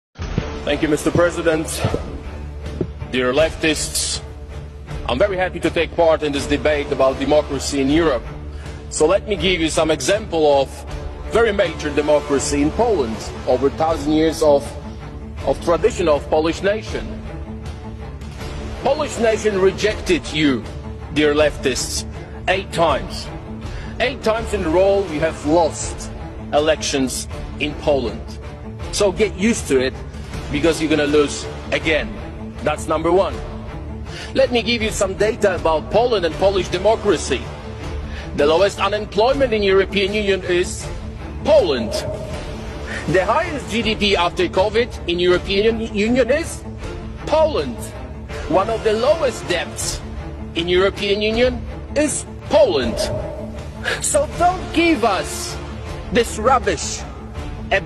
HE TELLS THE EU LIKE IT IS maga
As soon as he enters the auditorium and gets in front of the podium, he lectures the leftists in the European Union on what his Conservative government has done for Poland.